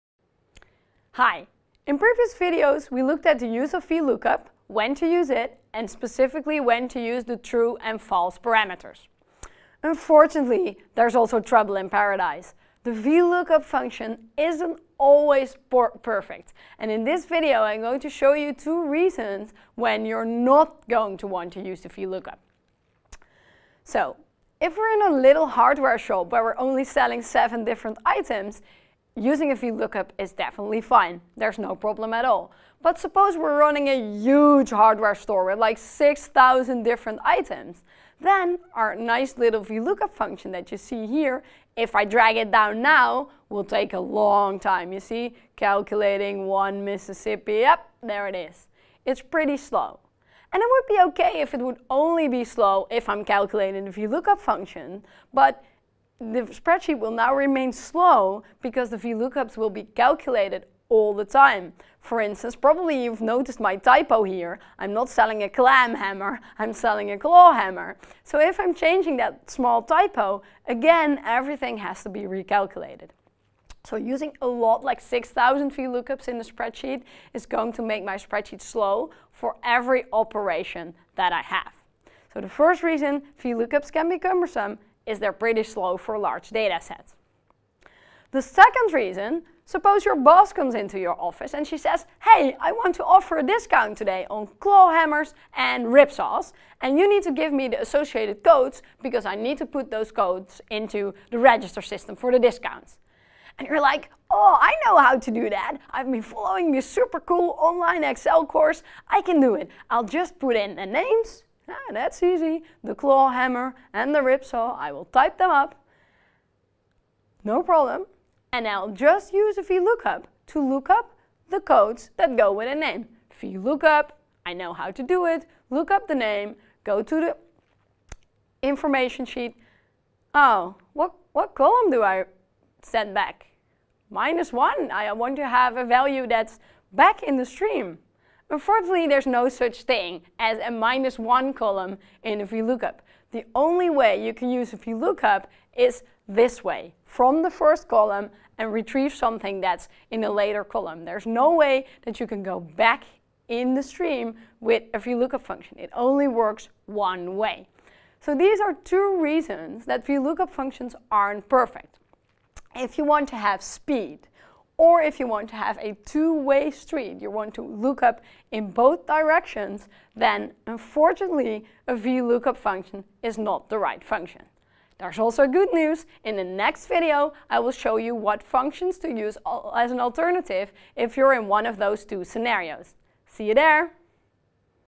Phân tích dữ liệu với Excel, Vietsub bởi kienhoc.vn - TUDelft EX101x - Tuần 2, Phần 5
Bài giảng của Tuần 2, môn Phân tích dữ liệu với Excel do trường TU Delft (Hà Lan) giảng dạy, phụ đề Vietsub bởi Kiến Học.

Đăng ký tại để học toàn bộ khóa học với đầy đủ bài kiểm tra, bài thi, cũng như được cố vấn khóa học giải đáp thắc mắc về nội dung khóa học.